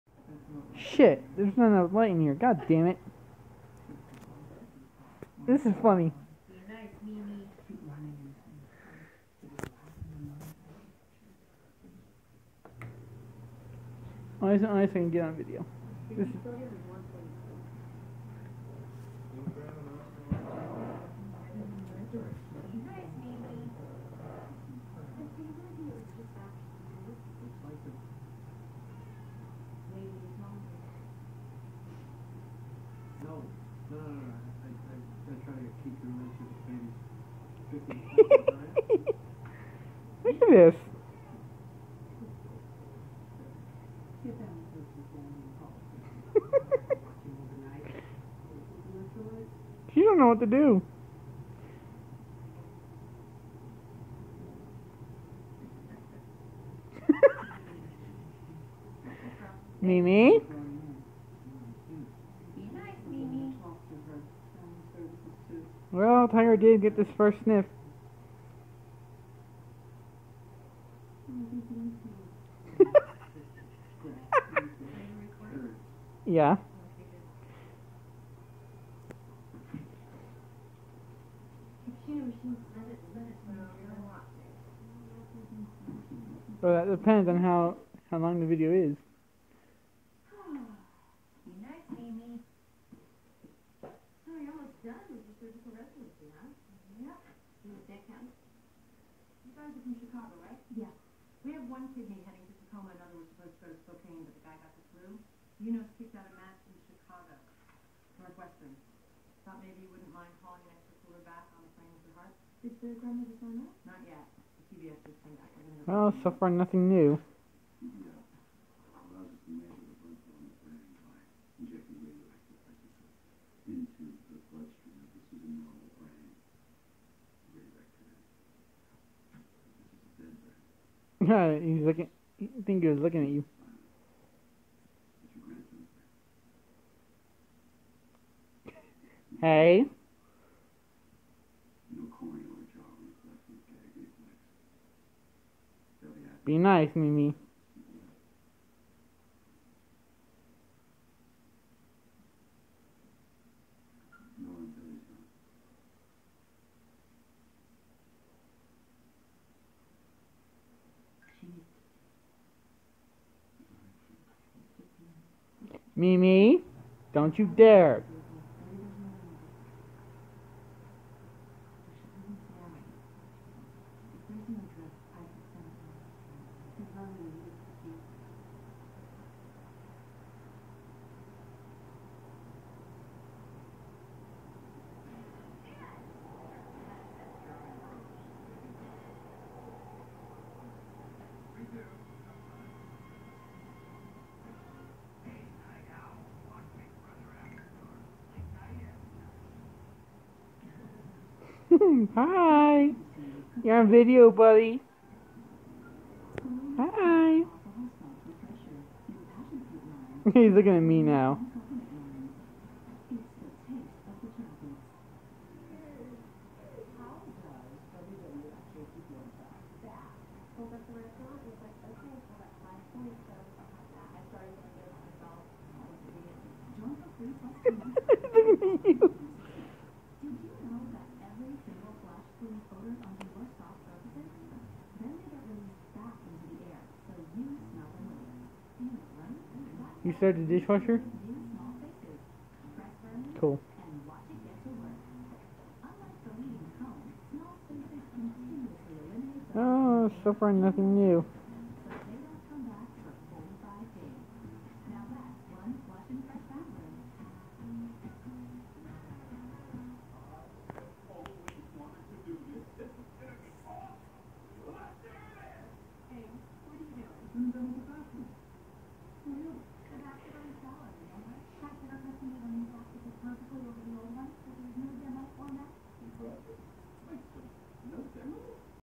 Tiger meets Aunt's Dog Mimi (2nd time)
Young Feline Tiger meets Canine Mimi for a 2nd time (1st time didn't get filmed), and Tiger tried to play with Mimi but Mimi started to bare her teeth more than once, and then nothing new happened after that.